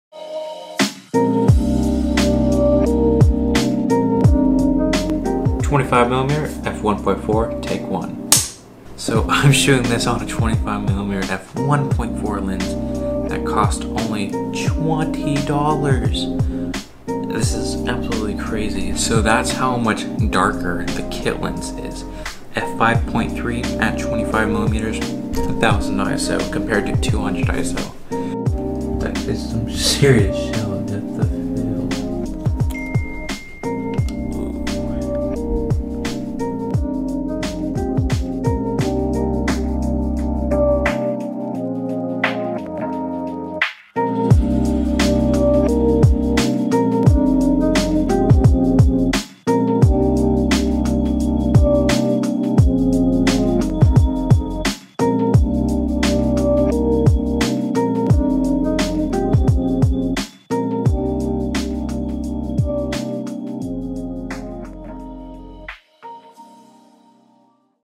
25mm f/1.4 C Mount CCTV Lens Test - Panasonic G7
Video test 25mm f/1.4 CCTV C Mount Lens on Lumix G7.
25mm f/1.4 C Mount Lens ➞
(may take a while to arrive, mine did)

The 25mm f/1.4 is a superb lens for the price of $20. There are a few drawbacks such as the flawed image, small size, and difficulty in operation. However it gives you artistic results and a f/1.4 aperture. :)